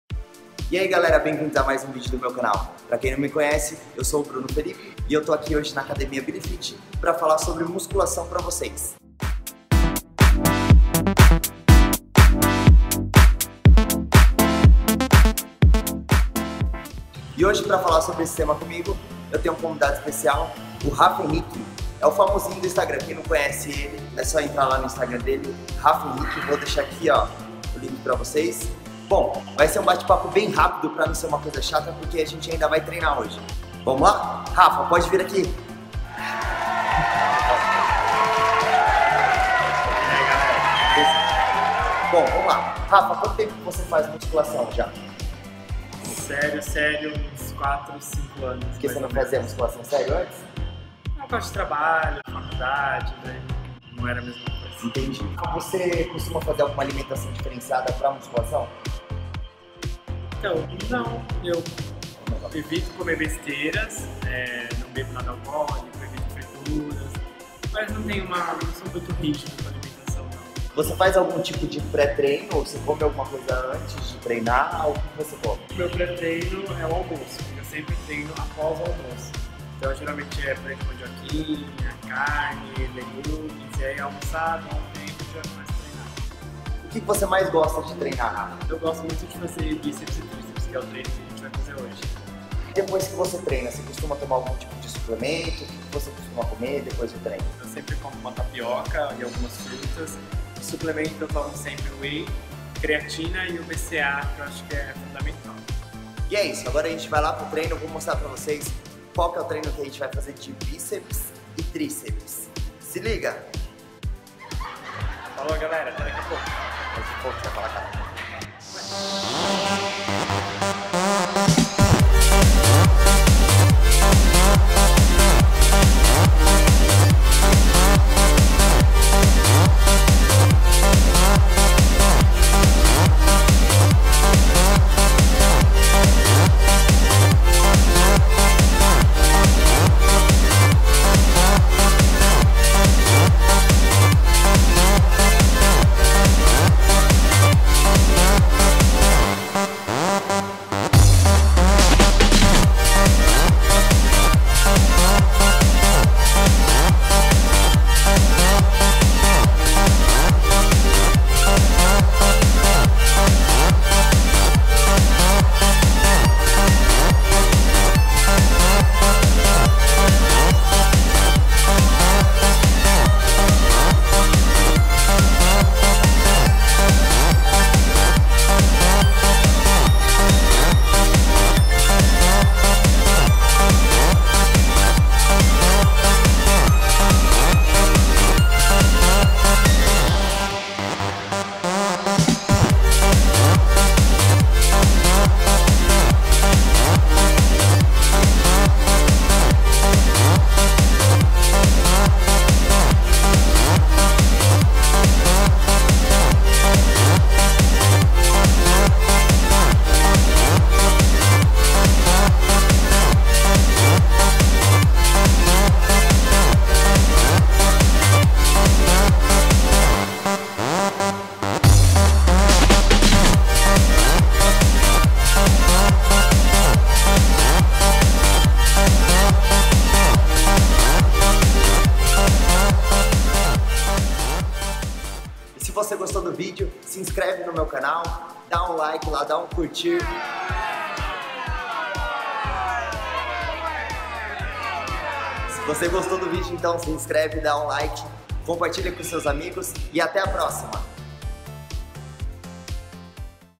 Treino Rápido de Bíceps e Tríceps com Rafaenrique
Hoje é dia de musculação aqui no canal galera!
Veja um treino super rápido e eficiente de Bíceps e Tríceps com o Rafaenrique do instagram!